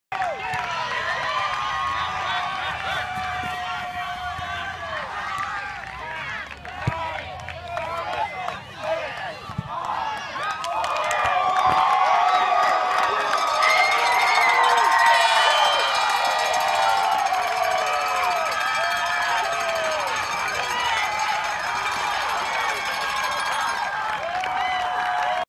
SOCCER: The final seconds tick off Saline's 1-0 victory over Brighton in the District 4 championship
By: Randy Castro - Sports Editor for Heritage Media-West.  Living the dream, one game at a time...